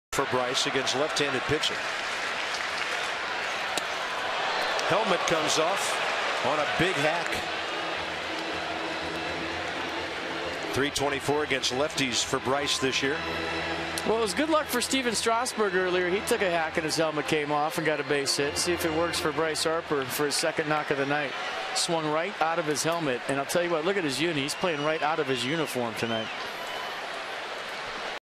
COL@WSH: Harper swings and misses, helmet flies off
8/8/15: Bryce Harper swings and misses a Christian Friedrich pitch, and his helmet flies off his head and rolls away